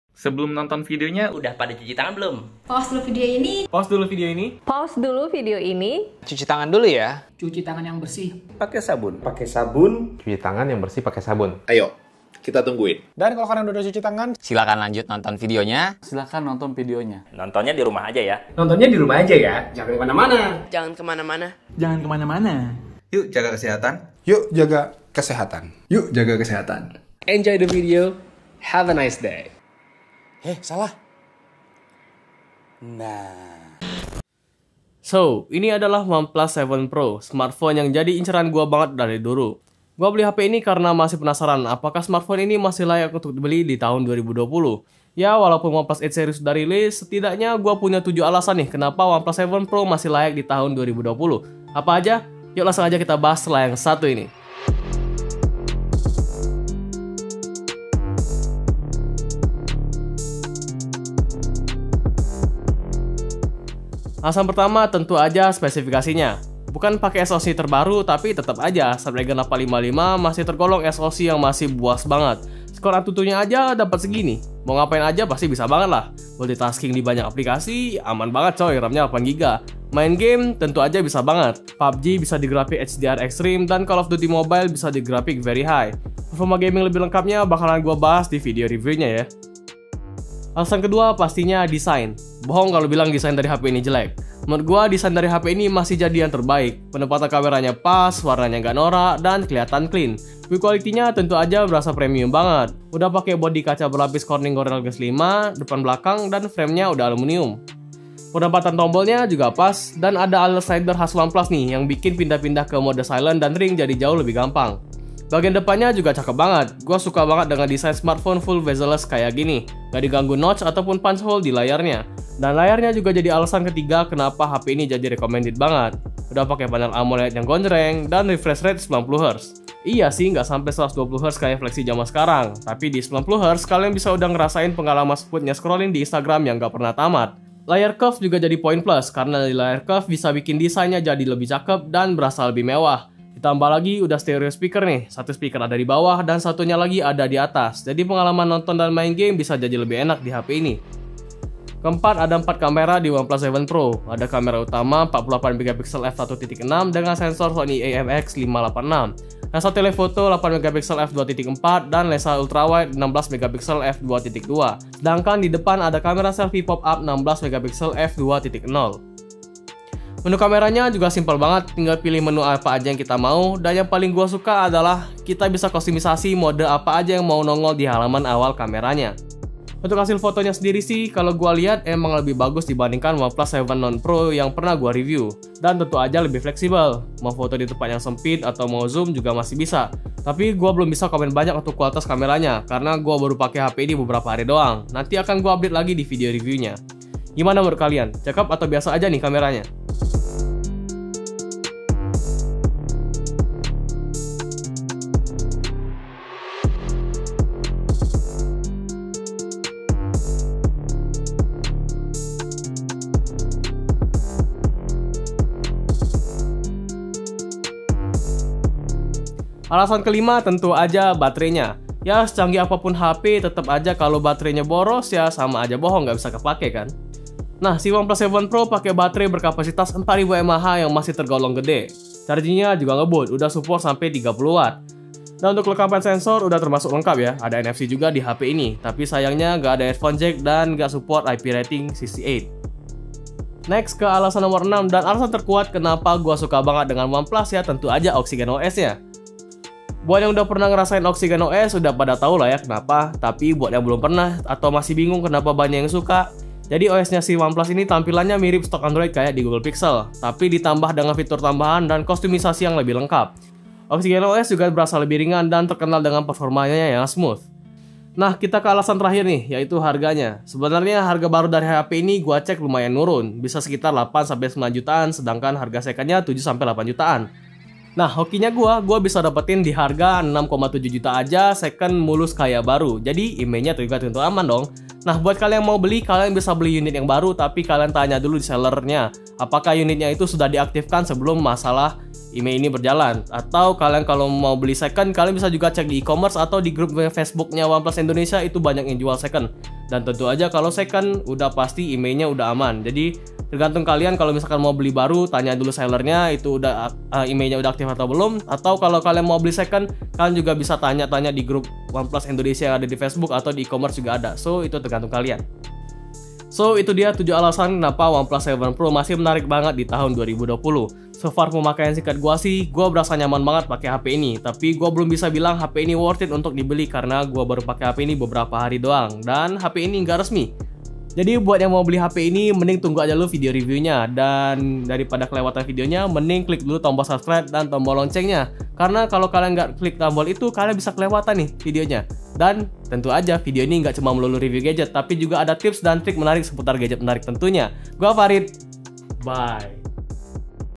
7 Alasan Oneplus 7 Pro Masih Menarik Banget di Tahun 2020
Itu dia 7 Alasan kenaapa Oneplus 7 Pro masih menarik
Gimana menurut kalian? Pengen beli juga? :D